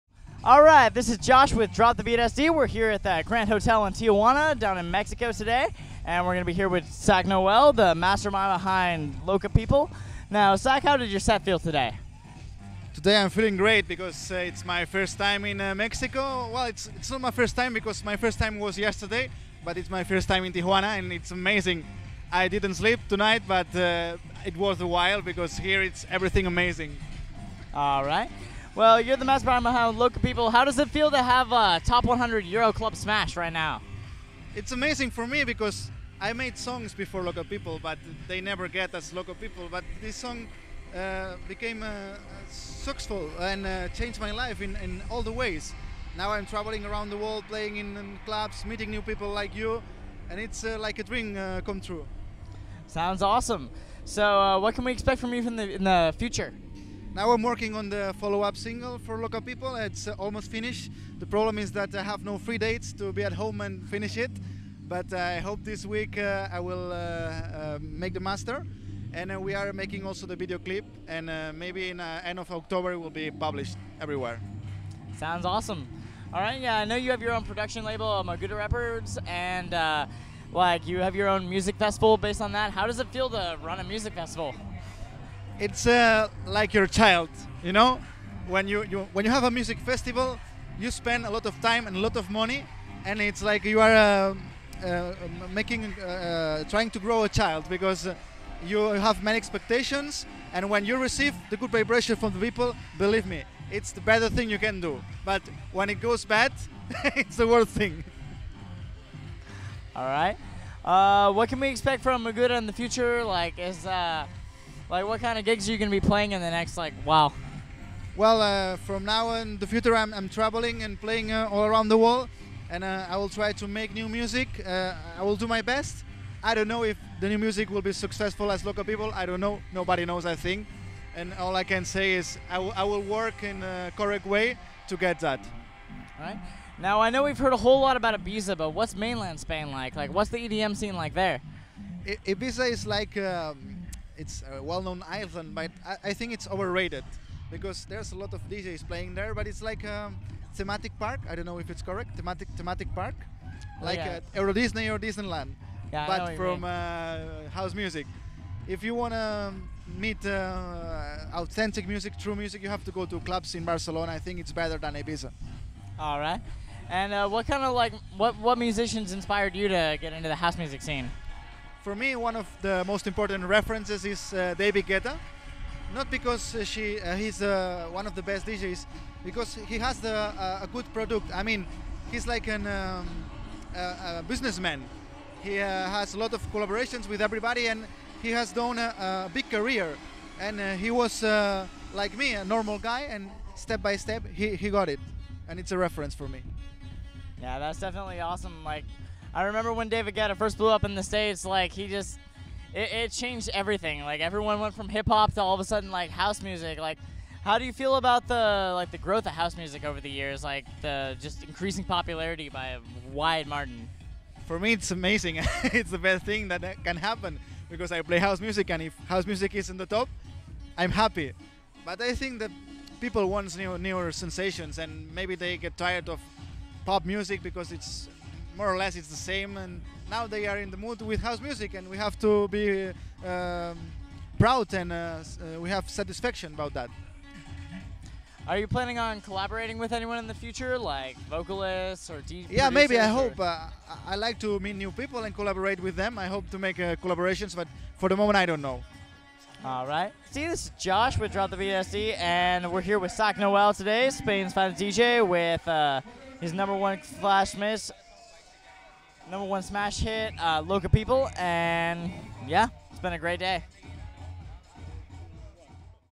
Sak Noel Interview | Grand Hotel Tijuana 09.24.2011
DropTheBeat met up with Sak Noel on September 24th, 2011 at Rehab Tj's Season Finale & Pavaj Events's one year anniversary at Grand Hotel Tijuana in Tijuana, Mexico.

Interviewed by Josh Hundley.

Thanks to Grand Hotel Tijuana and Pavaj Events.